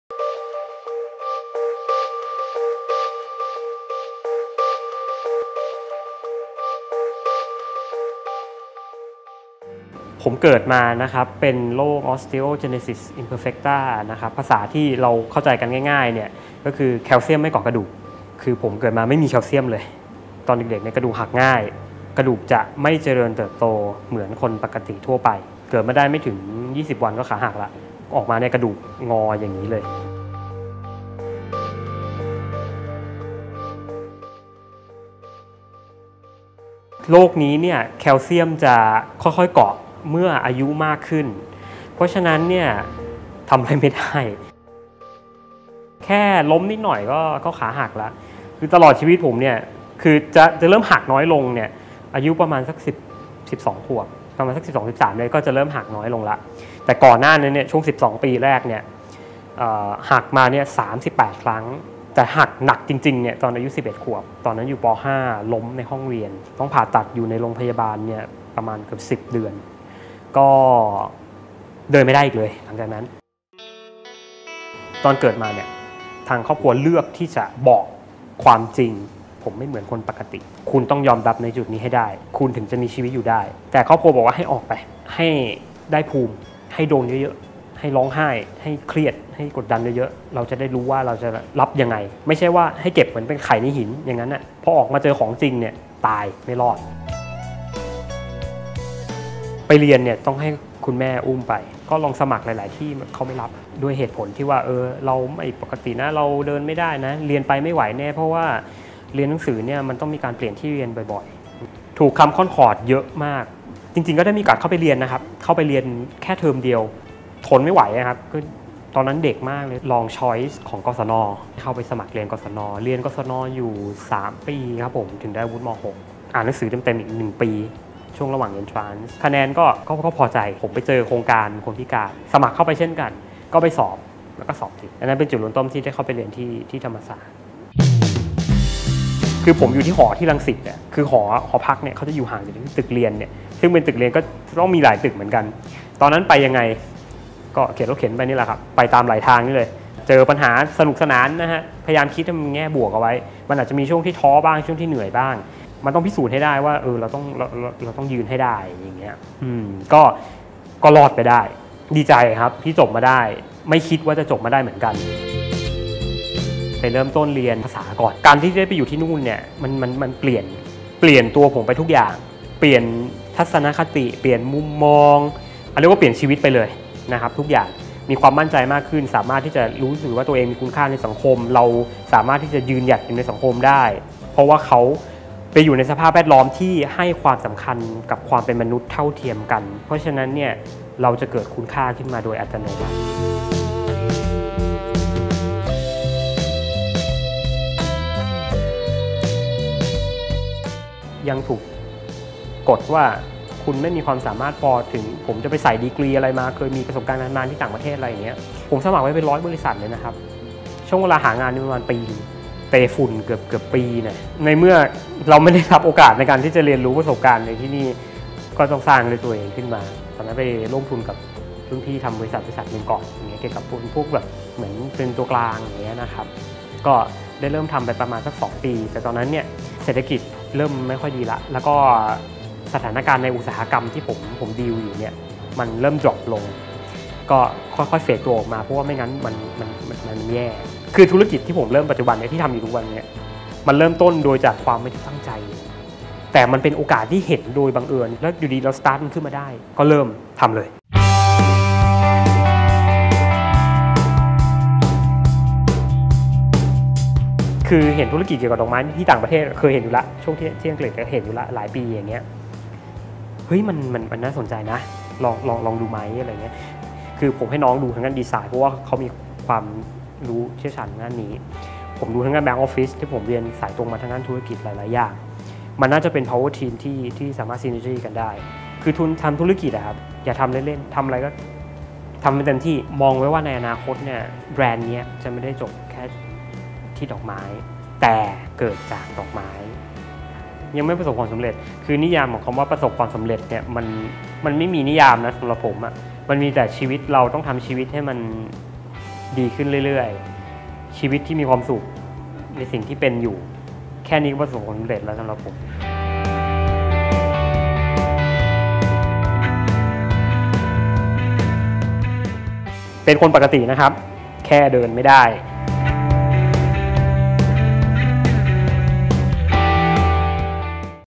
"ความพิการไม่เคยทำให้หยุดก้าวไปข้างหน้า" อภิพงศ์ ชูติกูล
หากคุณเดินไม่ได้ คุณจะใช้ชีวิตอย่างไร? นั่นคือสิ่งที่ "อภิพงศ์ ชูติกูล" ชายหนุ่มวัย 32 ปีเผชิญมาตั้งแต่แรกเกิด ชีวิตตั้งแต่เล็กจนโตมีอุปสรรคขวางกั้นมาต่อเนื่อง แต่เขาไม่เคยถอย เดินหน้าฟันฝ่าจนสามารถสร้างธุรกิจเป็นของตัวเองได้สำเร็จ